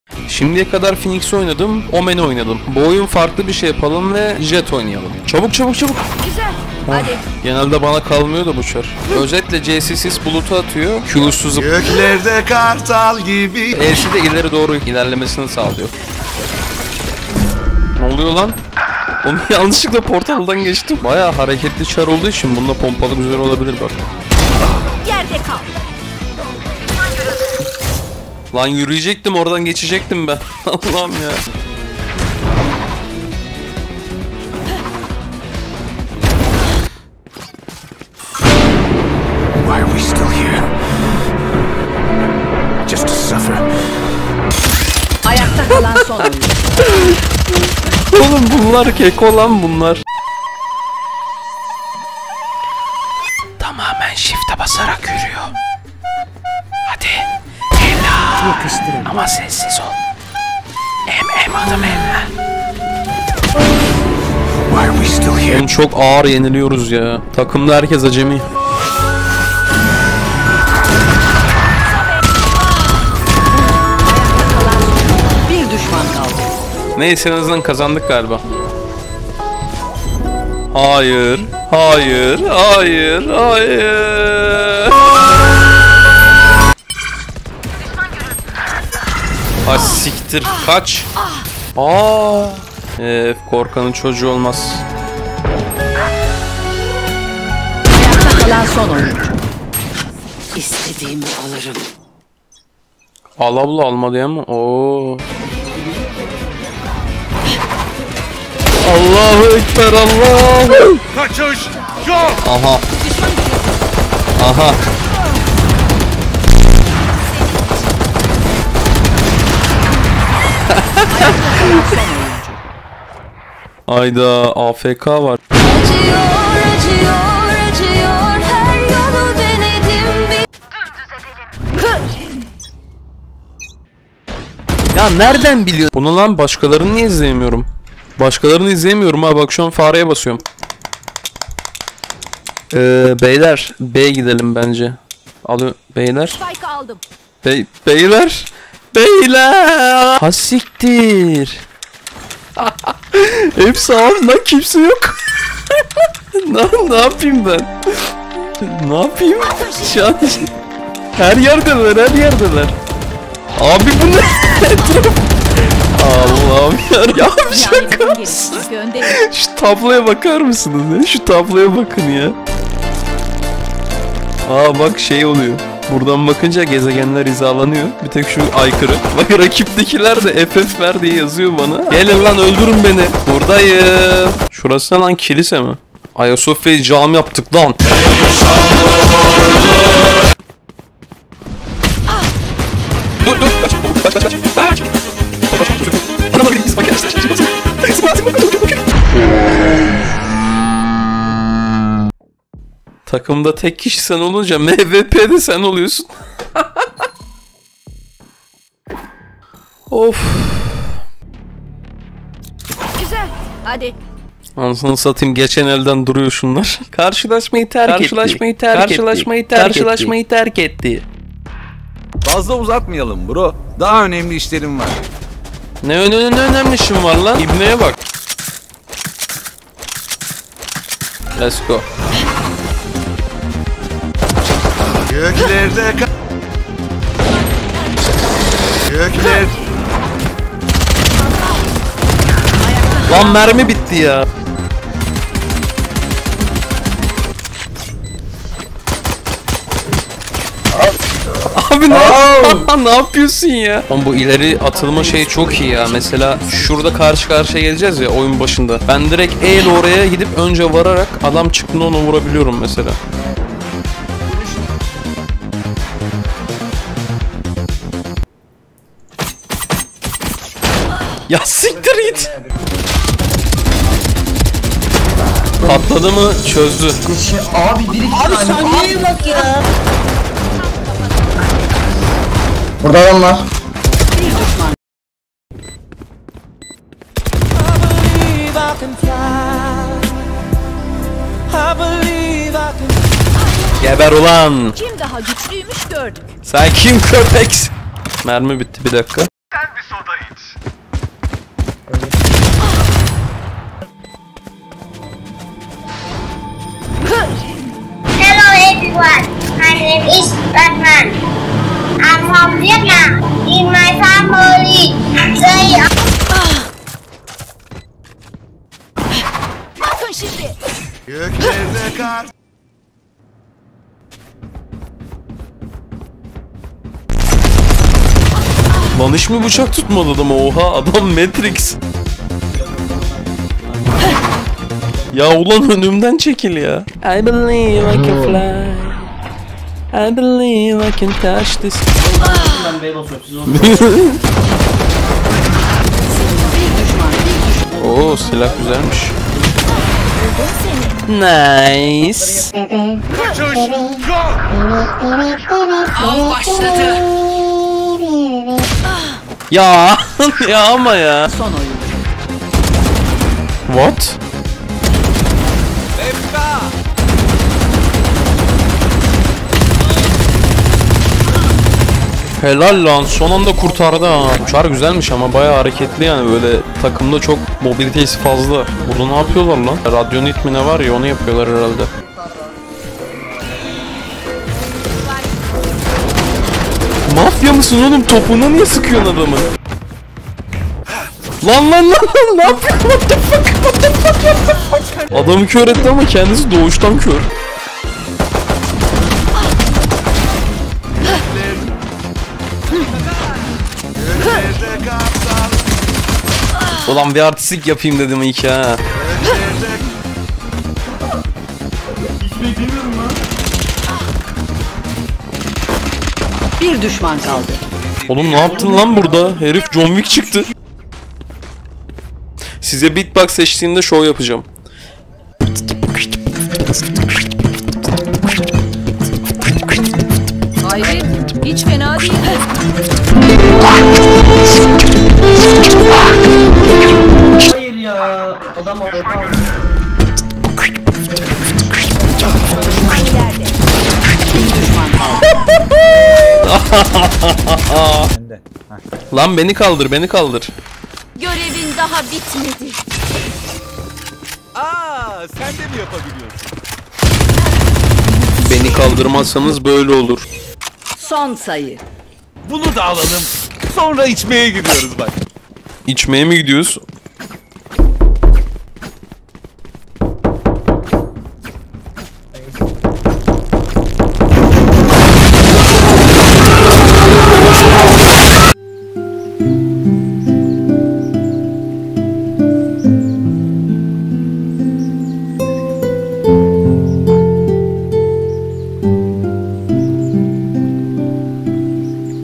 dört afk ama GÖKTE KARTAL GİBİYİM?
Atmosere yeni yeni ısınmaya başladığım sıralarda Jett ajanını oynamaya karar vermem ile ilk oyundan bütün takım arkadaşlarımın pes ederek AFK kalması sonucu 1 VS 5 kaldığım anlar; ve bir sonraki oyun hırs yaparak göklerde kartal gibi uçtuğum muhteşem görüntüler ile karşınızdayım.